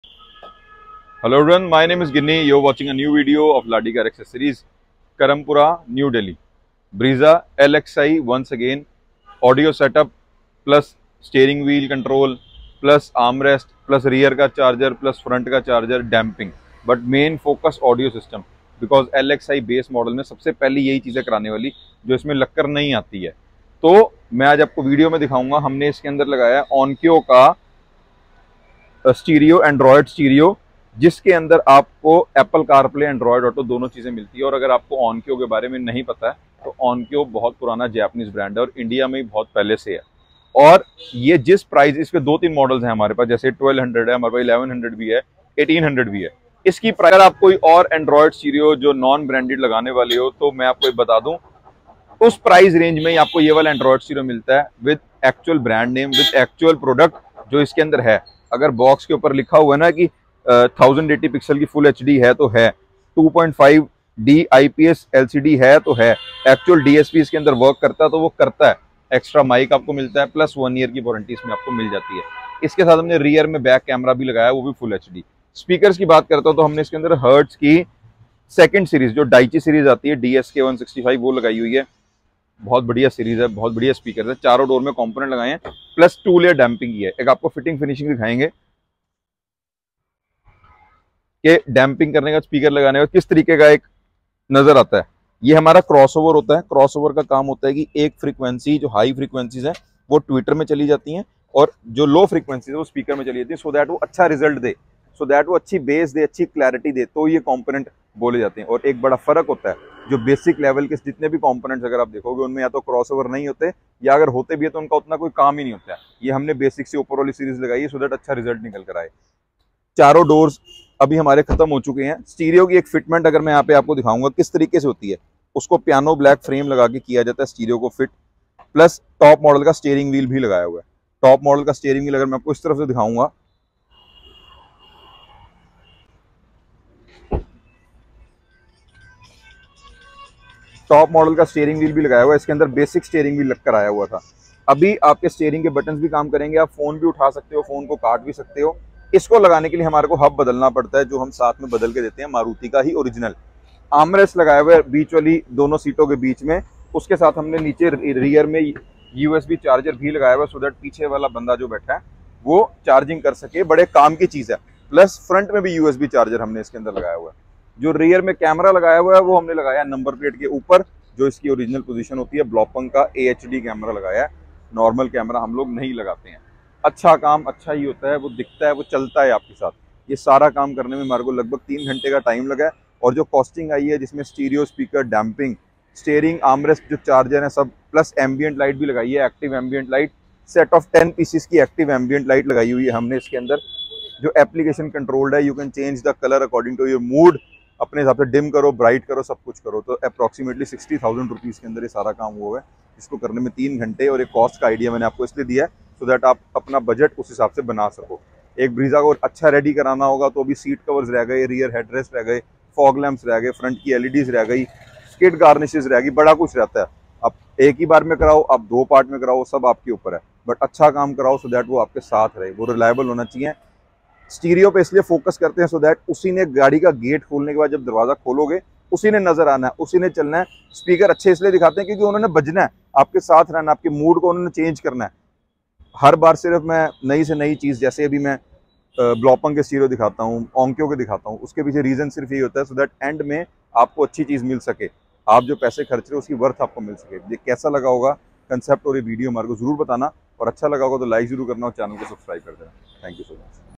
New brezza lxi modified✅new brezza base model music system✅onkyo android stereo✅brezza2023✅
Hi YouTube family in this video we have shown a budjet music system for new brezza

Produsts
Onkyo android stereo with 2/32 with Apple car play and android auto
Hertz component speakers
Rear camera
Door dampings

Ladi car accessories
I-191 Karampura main road Delhi -110015